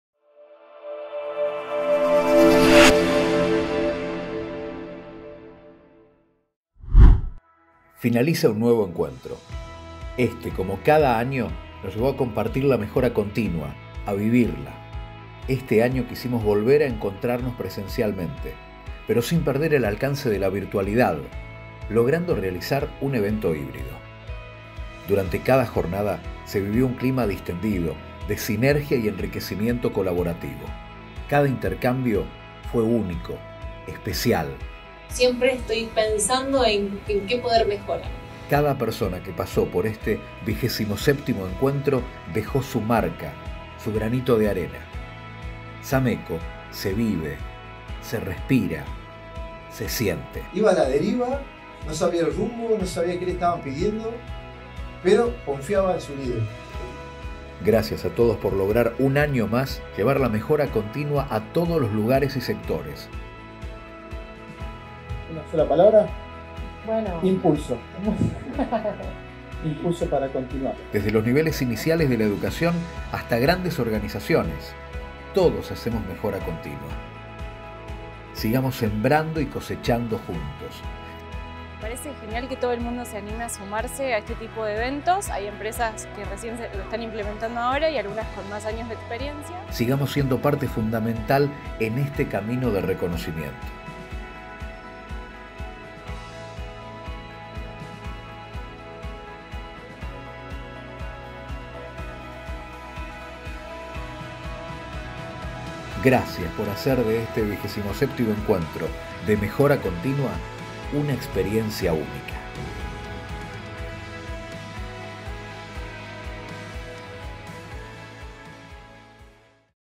27 Encuentro de Mejora Continua SAMECO 2022 Resumen
Finalizó el 27º Encuentro de Mejora Continua SAMECO 2022 en el que los autores de 28 Experiencias de Mejora compartieron sus vivencias y conocimientos, despertando el entusiasmo de más de 200 asistentes presentes en el Auditorio Central del INTI y de otros 500 que siguieron la transmisión online del evento. La gestión del cambio y la transformación digital fueron las temáticas sobre las que giraron las actividades. Reviví el 27° Encuentro y mirá los trabajos en la plataforma: experiencia.sameco.org.ar